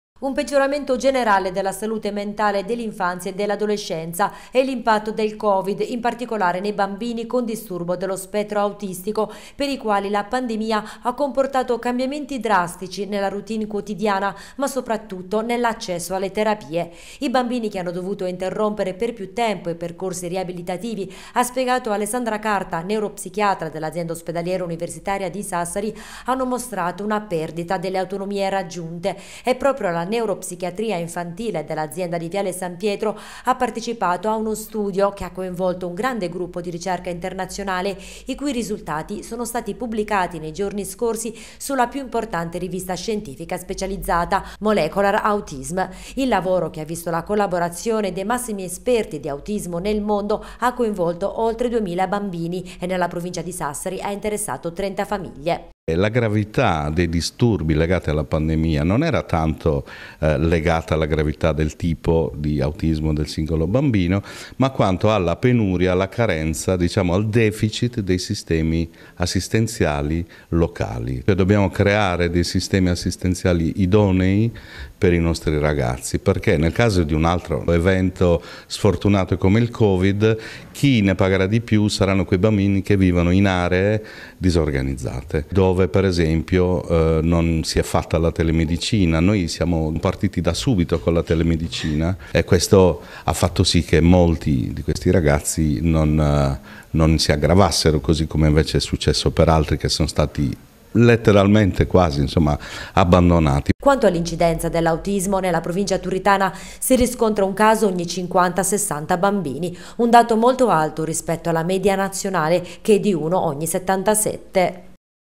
La Neuropsichiatria infantile dell’Aou di Sassari è stata protagonista di un studio internazionale
La Neuropsichiatria infantile dell’Aou di Sassari è stata protagonista di un importante studio internazionale sull’impatto del covid nei bambini e adolescenti in particolare su quelli affetti da spettro autistico.